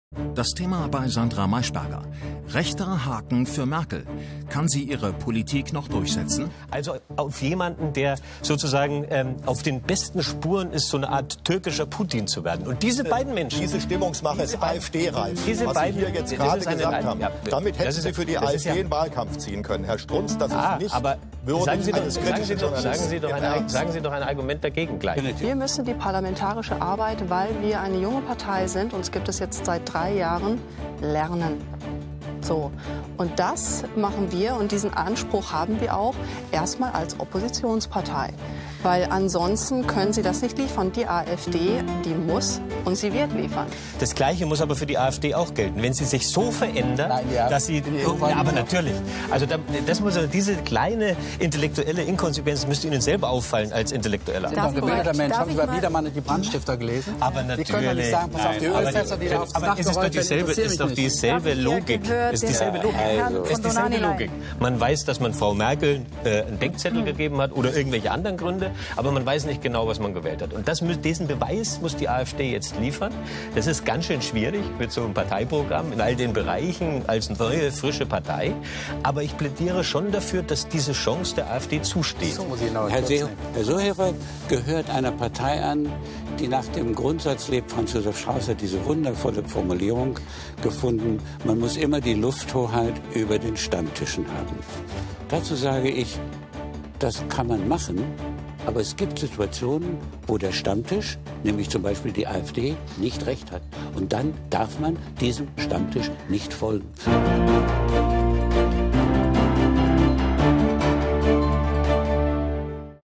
Highlights vom 16.03.2016
Ist die Kanzlerin als mächtigste Frau Europas politisch geschwächt – kurz vor dem Brüsseler Gipfel, kurz vor brisanten Verhandlungen zur Flüchtlingsfrage mit der Türkei, die Visafreiheit und beschleunigte Beitrittsverhandlungen zur EU fordert? In Deutschland haben die konservativen Wähler Angela Merkel am vergangenen Sonntag abgestraft, sind scharenweise zu den Rechtspopulisten übergelaufen. Heißt das auch: Die nationalistischen Gegner der Kanzlerin in Europa können sich gestärkt fühlen und Merkels Kurs boykottieren?